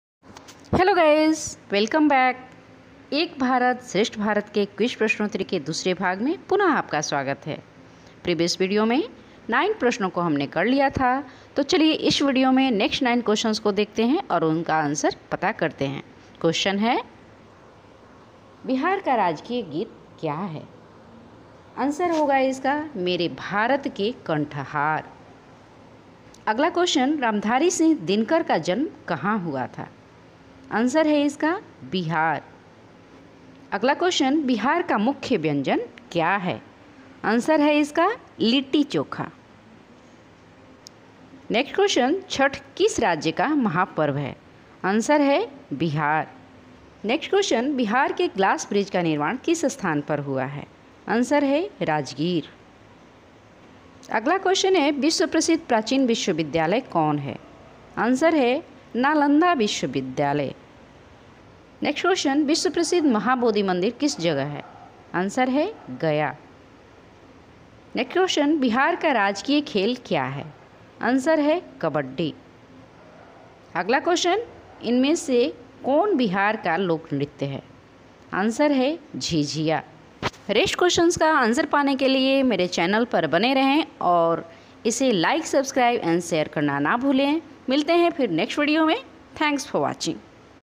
एक भारत श्रेष्ठ भारत क्विज 2021,Part-2 बिहार, त्रिपुरा और मिजोरम राज्य के विविध संस्कृति की जानकारी
Ek Bharat Shreshtha Bharat Quiz 2021

एक भारत श्रेष्ठ भारत कार्यक्रम के अन्तर्गत बिहार शिक्षा परियोजना परिषद, पटना द्वारा एक वृत चित्र (Documentary) एवं क्विज़ तैयार किया गया है ।
Documentary के माध्यम से तीनों राज्यों के समृद्ध व विविध संस्कृति के बारे में जाना जा सकता है, साथ ही क्विज़ में भाग लेकर प्राण पत्र प्राप्त किया जा सकता है ।
क्विज़ में भाग लेने के लिए विजिट करें -

Face book Account  - Hamara Vidyalay